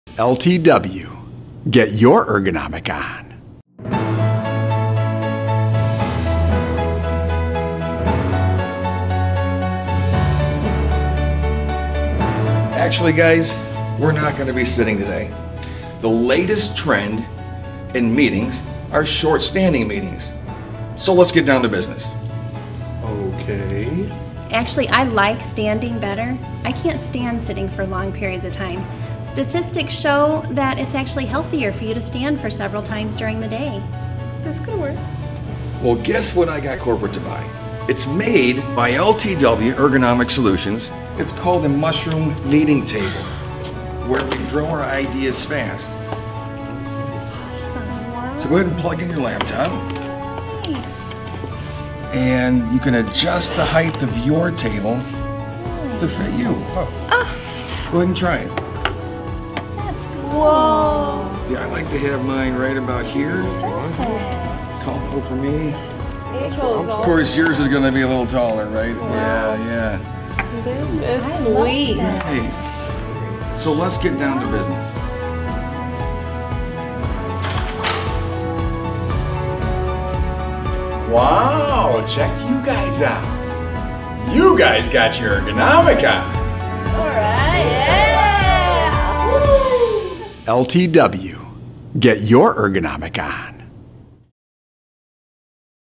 Height Adjustable Meeting Table - LTW Ergonomic Solutions
Anywhere you find people coming together for short standing meetings, you will start to see these new Patent Pending MConference tables growing up to fill the demand! This table is where you can “Grow Ideas Fast”! Each of the four table sections can easily be adjusted in height with the touch of a button so that everyone who comes to this table can work safely, efficiently and comfortably. With LTW’s custom configurations, sizes, privacy panel heights, colors to match your décor, and charging ports at each section, you will see these popping up everywhere!

LTW hopes to introduce your company to a new concept. Who likes long meetings with no end in sight? Not our company! With the MConference Table, coworkers can simply meet up at a table, carry on a discussion, and end it off before anyone gets too tired of standing. With plenty of room for laptops, no one will gripe at the thought of another meeting. Your coworkers may or may not be short or sweet- but with the MConference Table, all meetings will be short AND sweet!

Standard MConference Table Features include the following:
Quiet Electric motor driven column lifts at each of the four stations
Laminated Table tops
Steel guard panels
12 in. Motor Stroke height adjustability
120 VAC and USB Charging ports at each section
Solid Aluminum Base Plate w/ Floor Levelers
Universal Central Mounting Post for optional accessories
Our MConference Tables can be customized to fit your specific needs, which can include the following:

Custom Configurations:
Custom Colors
Custom Sizes
Table Top Options (Different colored laminates, solid hard woods, aluminum, bamboo, custom)